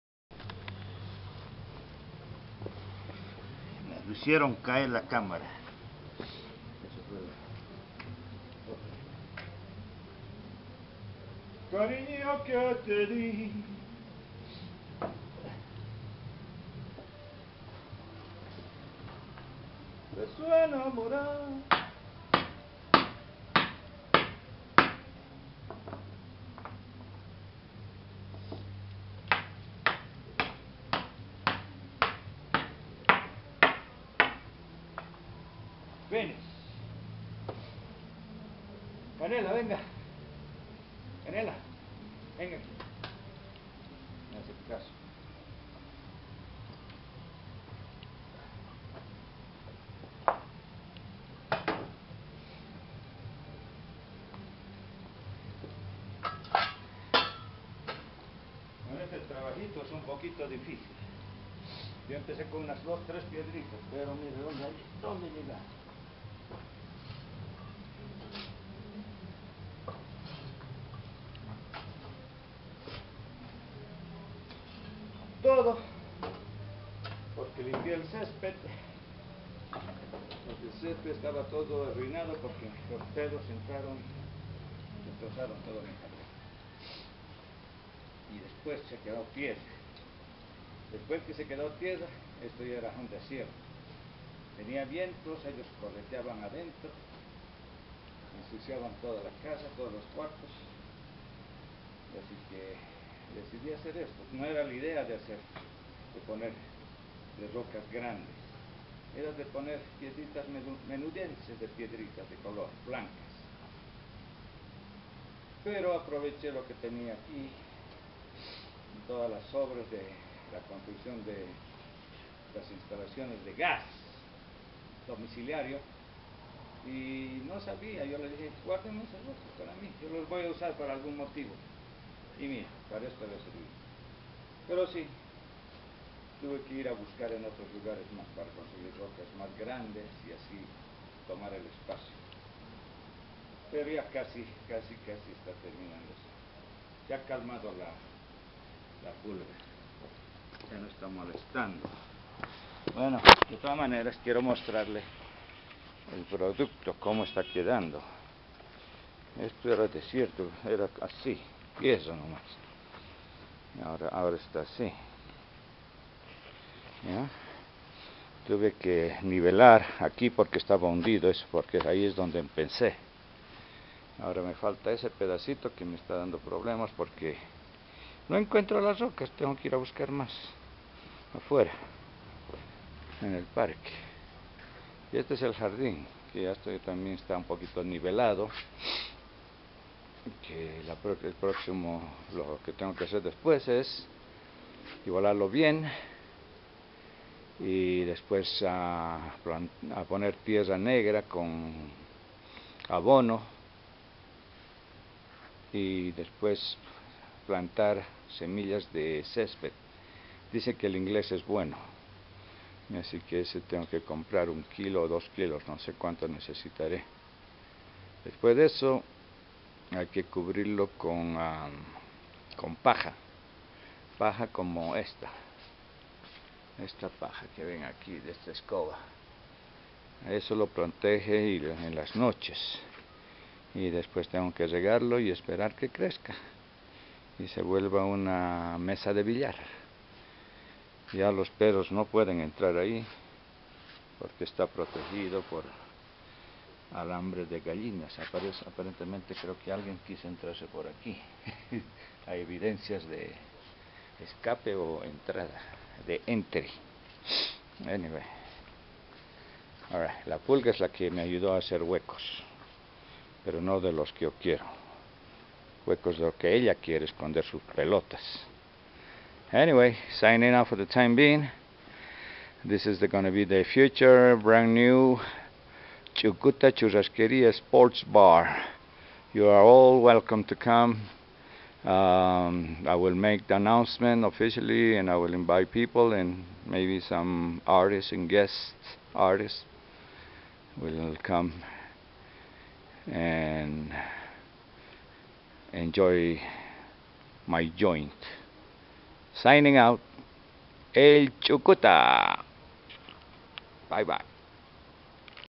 Empedrando parte de mi jardin.
Divertido empedrando . Me puse una chamarra para trabajar de mi papi sin darme cuenta y la realidad me ayudo a terminar el empedrado. Thank you Papi.Mi papa ( QEPD)